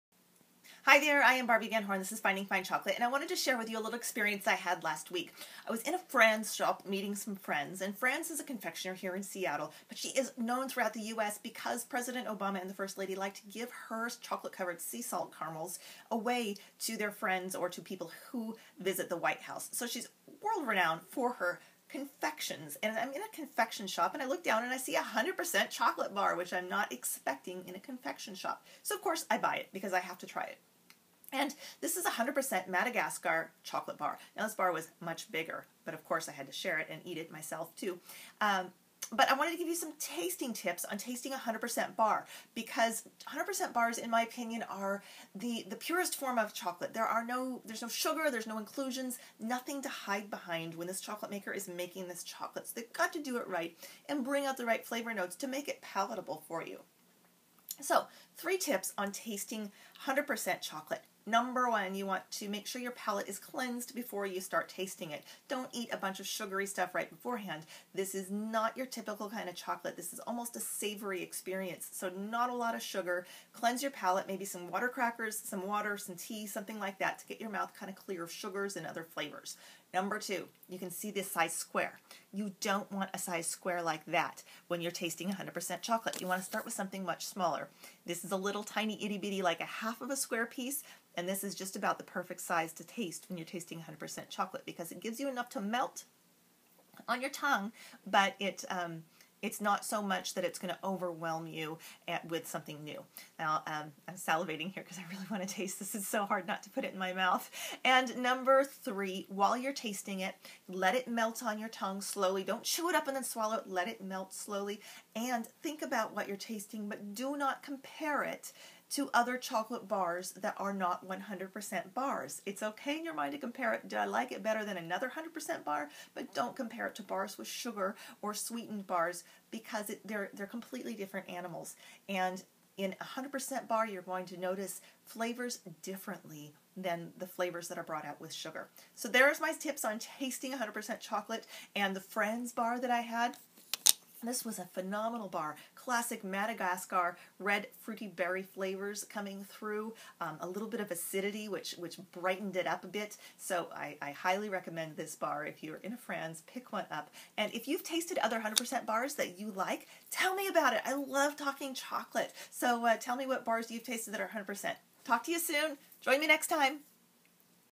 Tasting Tip: Fran's 100% Madagascar Bar. No sugar, all chocolate.
Tasting a 100% bar is a bit different than a sweetened chocolate bar.  I'm offering a few tips, or guidelines I follow, and how I found this Fran's 100% Madagascar bar.